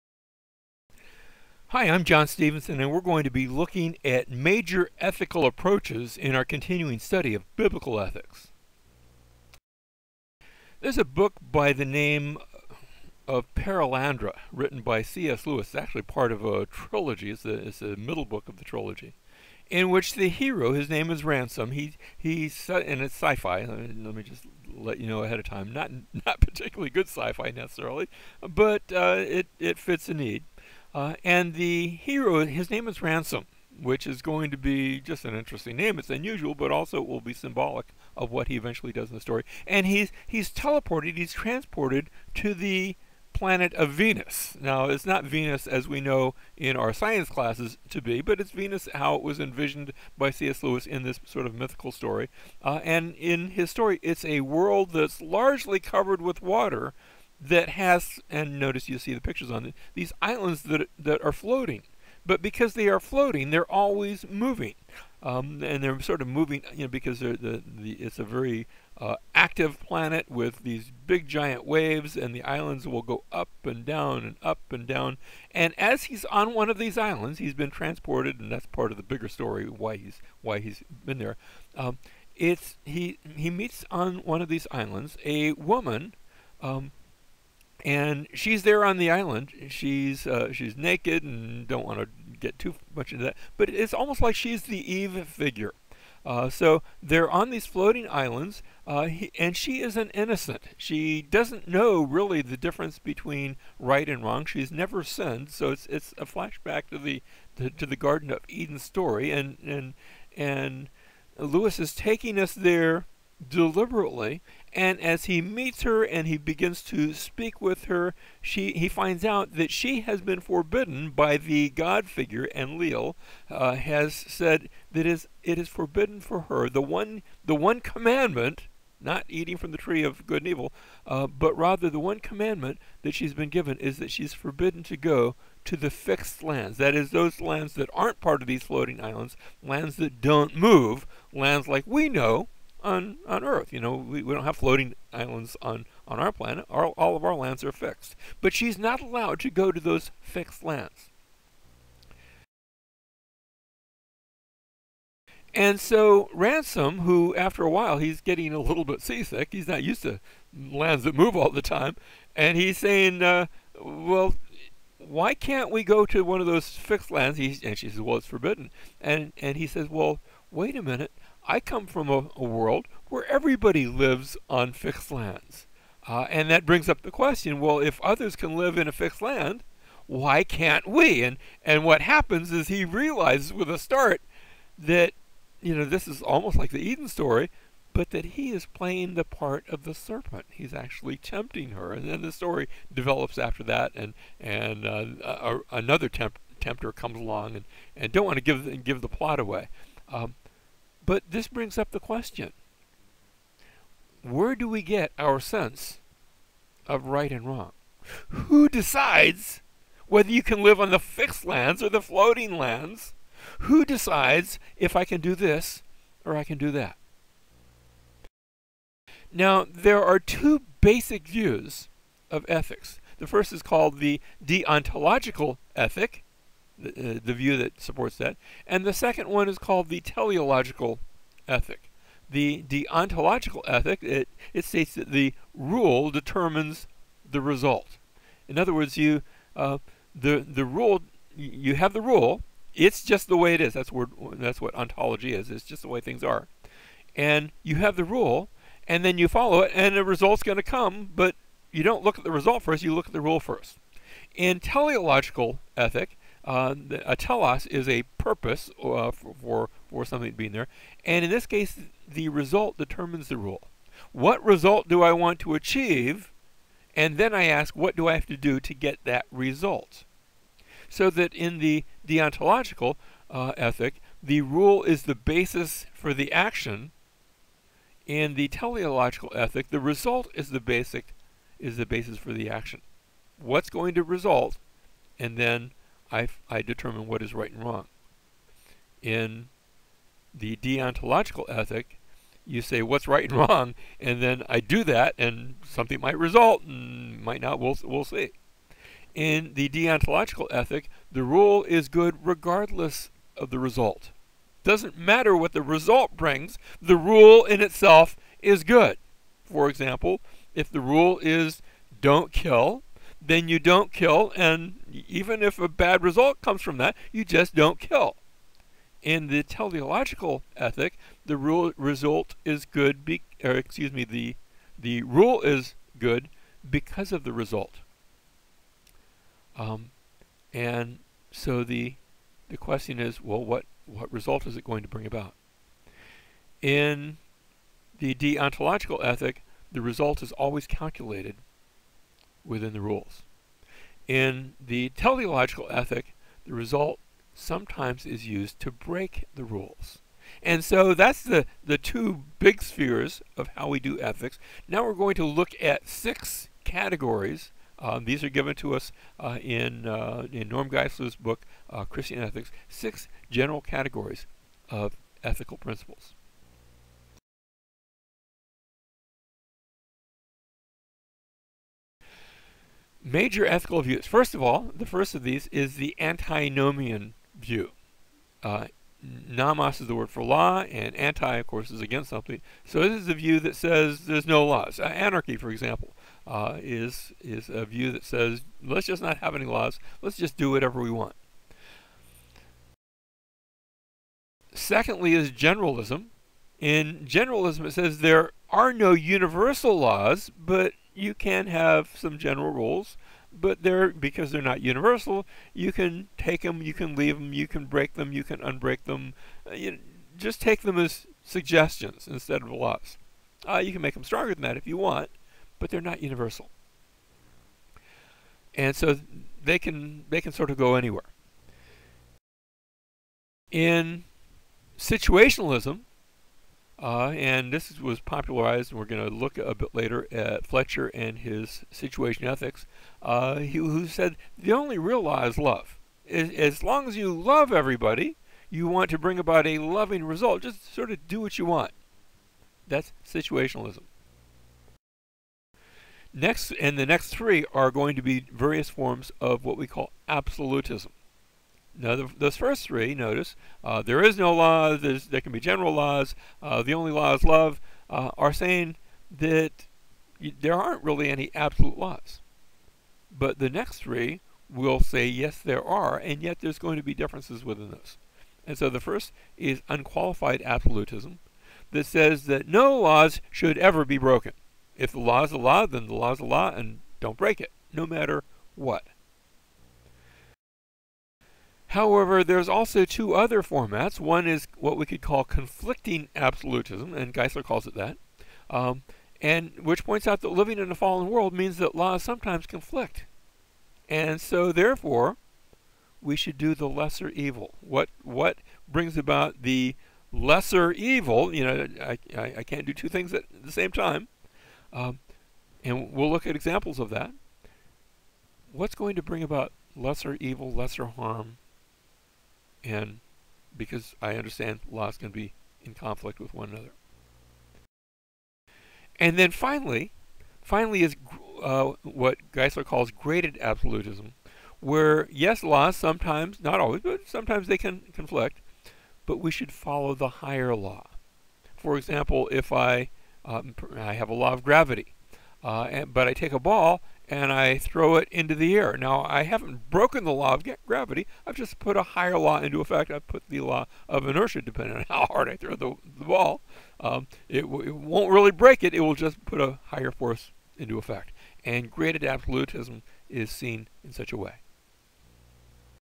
Biblical Ethics 4: Major Ethical Approaches
A brief introductory survey to the six basic ethical approaches.  This is part of a larger series on Biblical Ethics to be found – this series was developed for a course at South Florida Bible College.